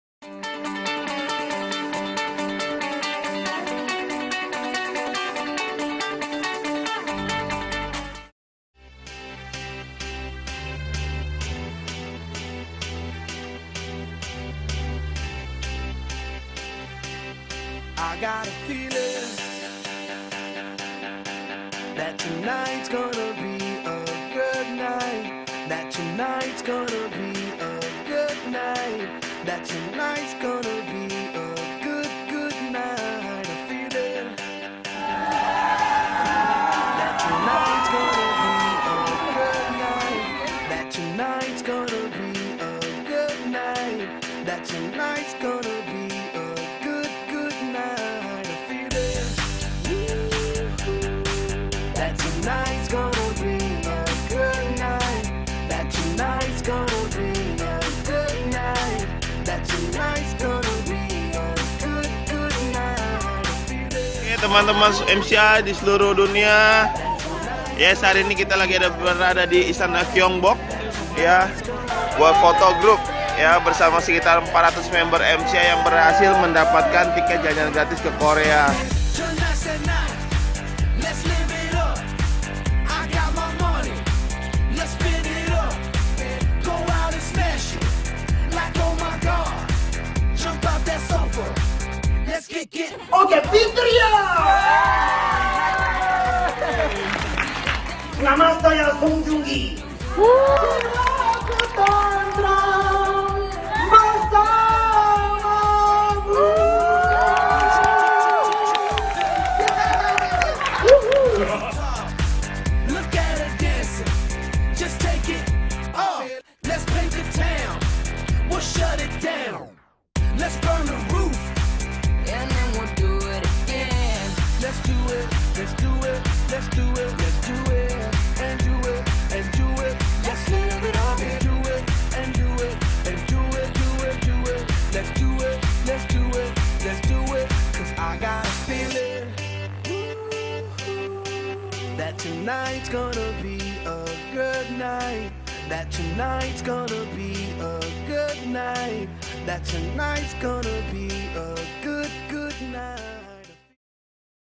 TRIP KOREA MCI 2017
Video Slide Foto-Foto para member yang disambut dengan baik oleh Departemen Pariwisata Korea dan keseruan kegiatan mereka selama Liburan.

Sampai diliput oleh KTO Jakarta dan YTN News loh...

Pssstttt. Video ini cuma berisi foto-foto dari Kloter pertama dari 8 kloter yang berangkat Liburan ke Korea bareng MCI loh...

Keren kan...